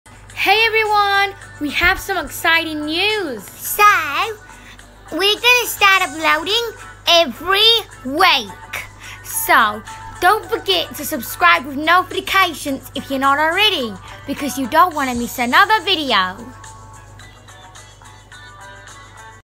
uploading days will be…
I’ve decided what day I’m gonna make new episodes! A new one will be coming every week!